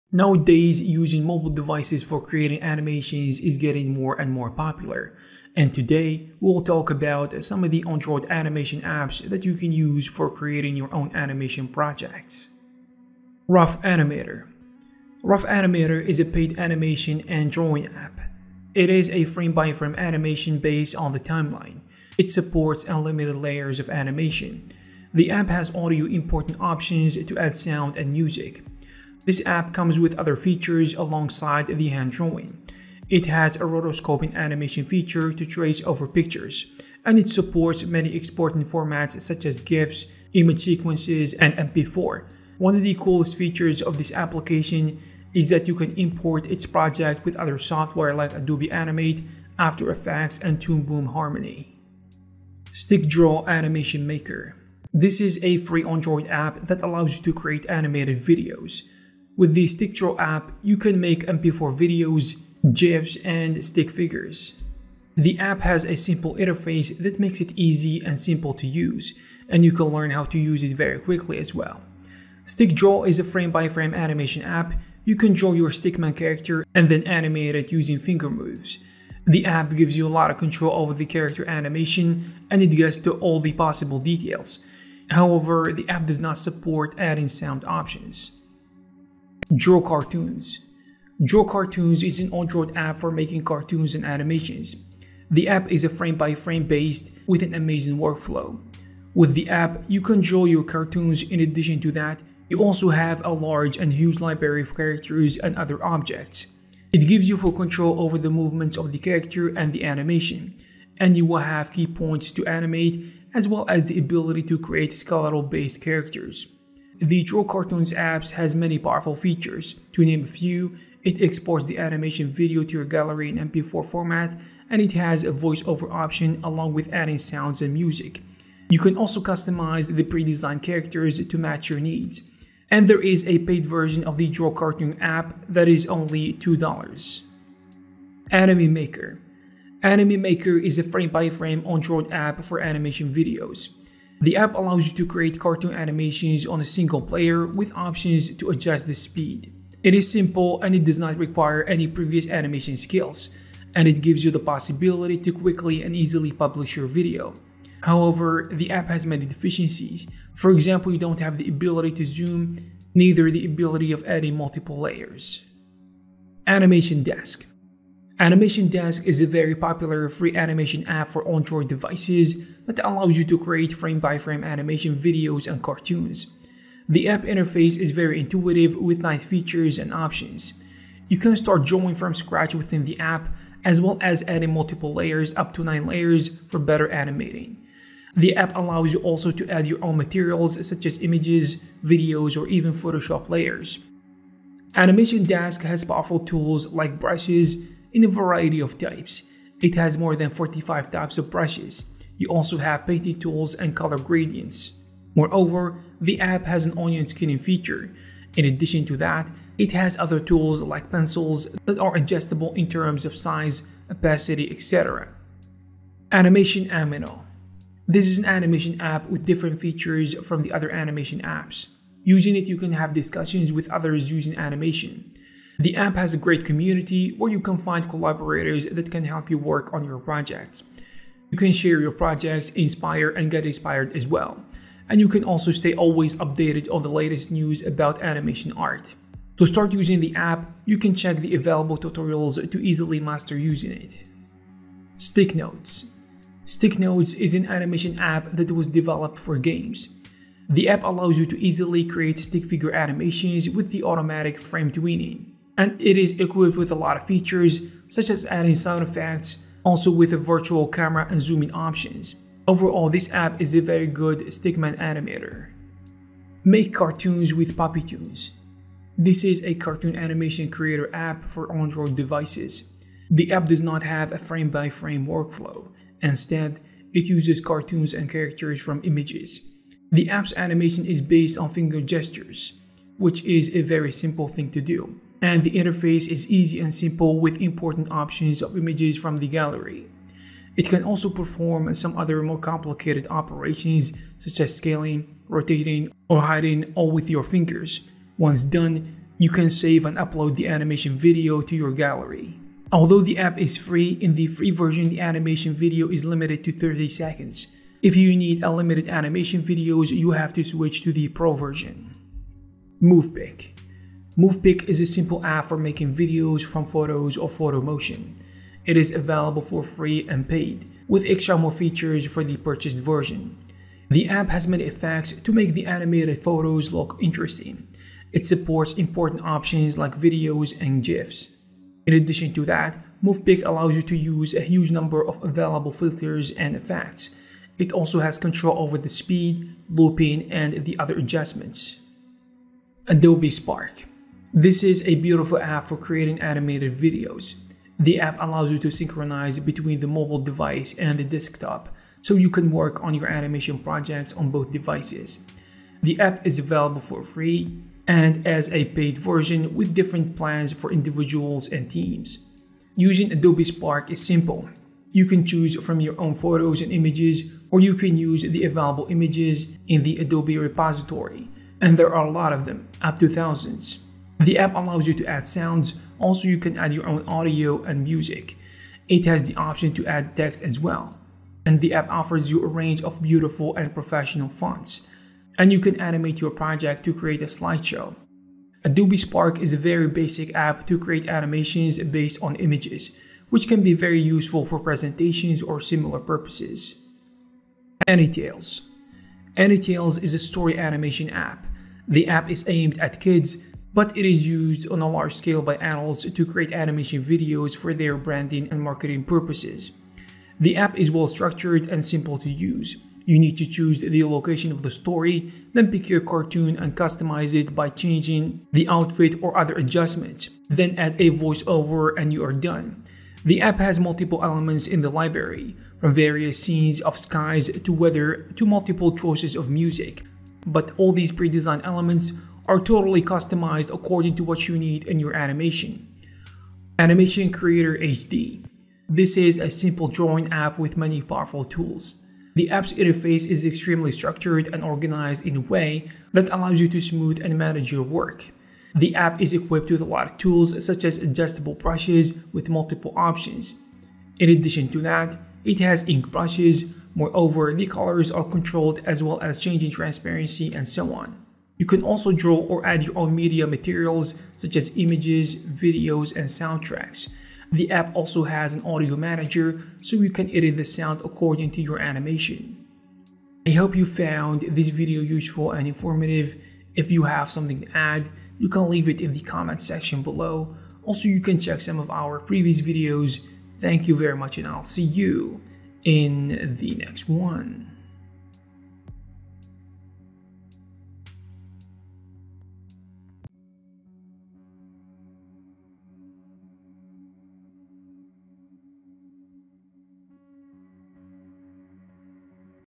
Best 2D Animation Apps for Android Devices
Nowadays, using smart mobile devices for creating animations is getting more and more popular. and Today, we will talk about some of the Android animation apps that you can use for creating your animation projects.